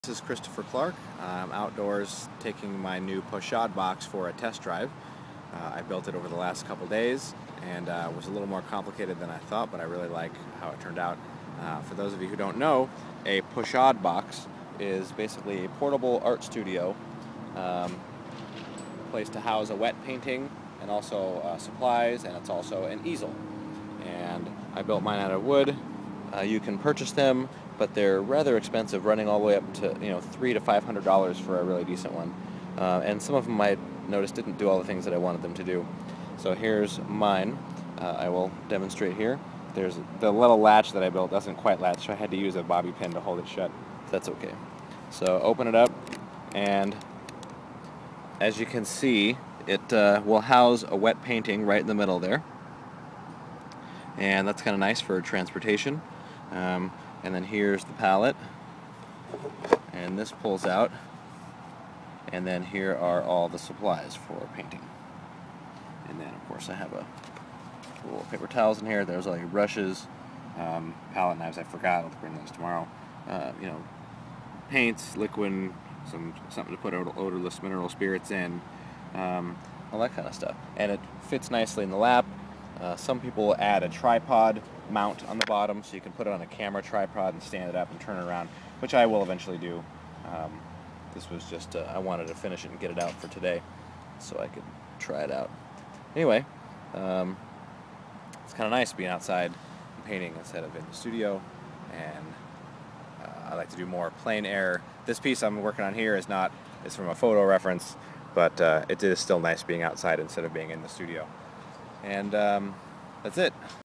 Home made pochade box - Christopher Clark, fine art
Painting en plein air ("outside" or "out in the open") introduces the need for a pochade box of sorts. It's a portable easel, supply storage, and wet painting storage. They can run from $300-$500 for a decent one, but I built my own for about $50 from stuff you can buy at any home improvement store. This is my variation on the Serrett pochade box created by Jim Serrett. Thanks for your ideas and videos to everyone else who have made theirs as well, they were very helpful. I'm happy to share my plans if anyone would like them.